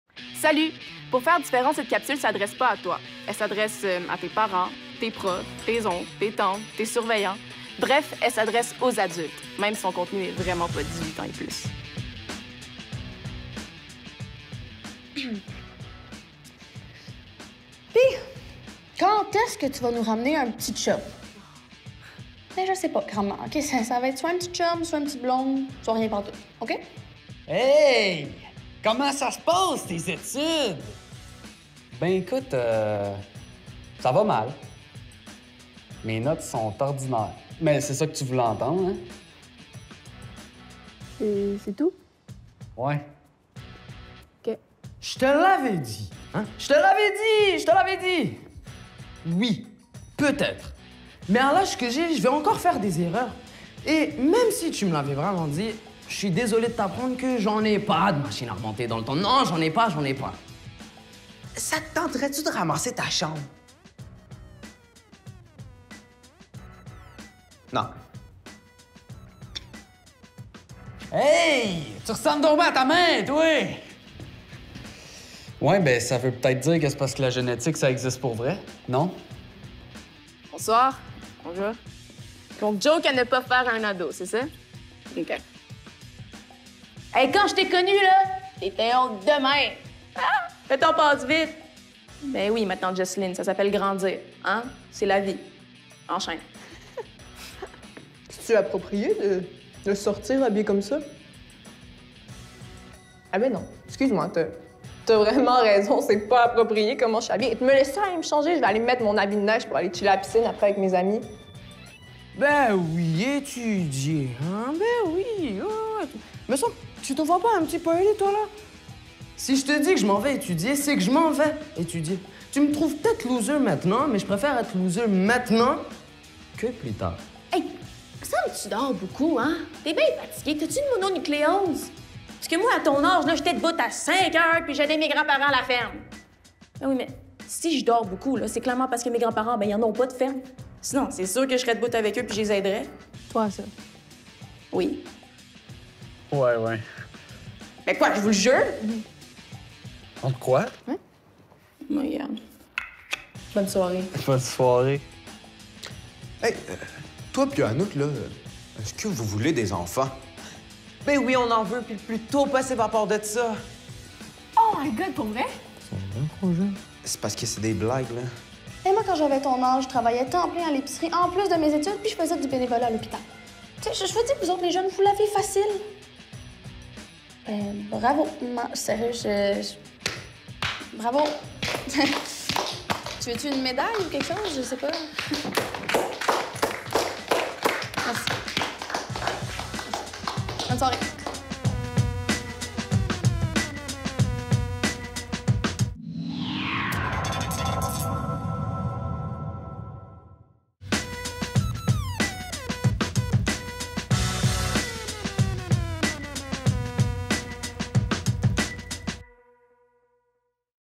Les pires phrases à dire à un ou une ado
Voici ce qu'il faut répondre aux réflexions pas rapport de nos parents, mononcle, profs... Car nous aussi, on peut en avoir assez d'eux!

La saison 4 c'est tous les vendredis à 17h sur ICI TÉLÉ et gratuitement sur ICI Tou.tv